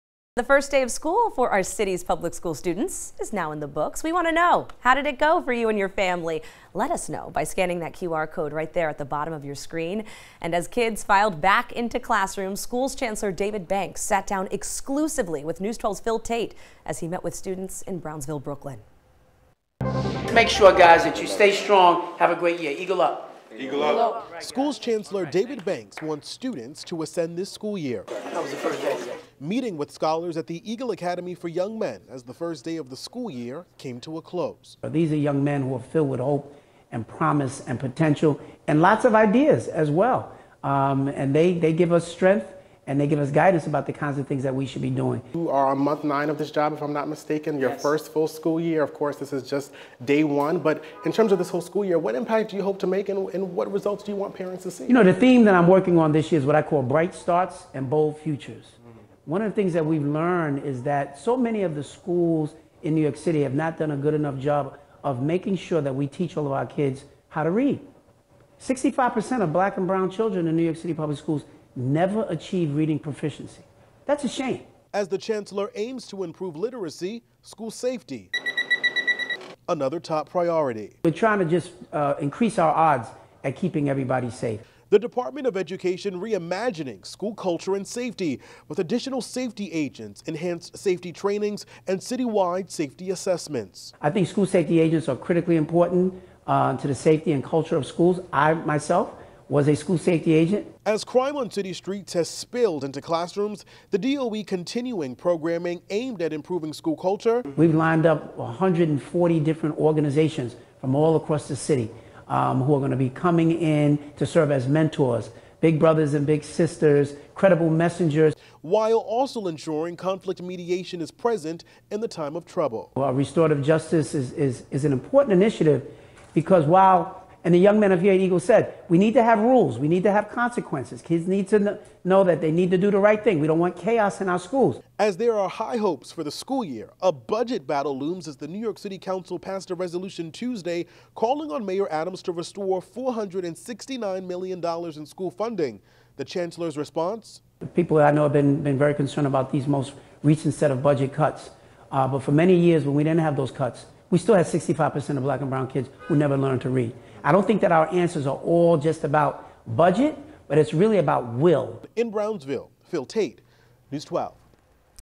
DOE Chancellor Banks closes first school day at Eagle Academy
Banks spoke with young men of color at the academy about their hopes for the future and what they would like to see in their educational journey.